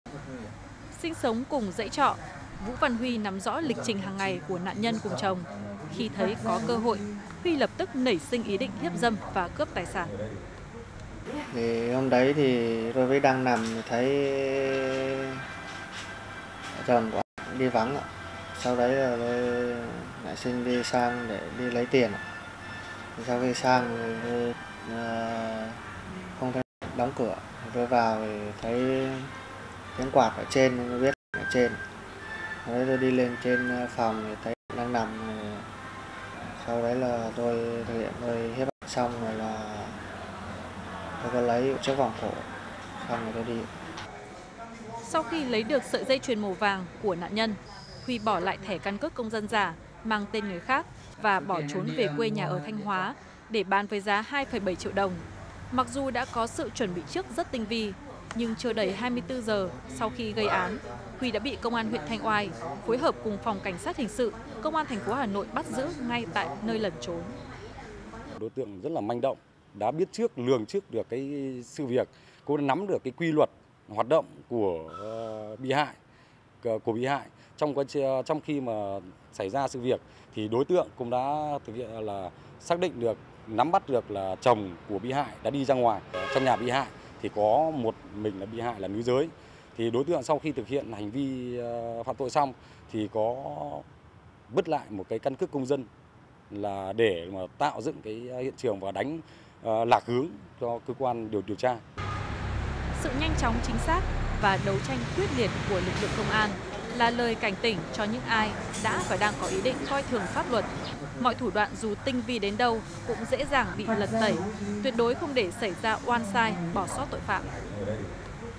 Bỏ lại căn cước để đánh lạc hướng điều tra | Bản tin 141
Sau khi tiếp nhận tin báo, Công an xã Cự Khê cùng Đội Cảnh sát điều tra tội phạm về trật tự xã hội huyện Thanh Oai đã bắt giữ Vũ Văn Huy, xã Hoạt Giang, huyện Hà Trung, tỉnh Thanh Hóa về tội hiếp dâm và cướp tài sản. Đối tượng này không chỉ có 4 tiền án mà còn sử dụng tinh vi nhằm đánh lạc hướng điều tra của cơ quan công an.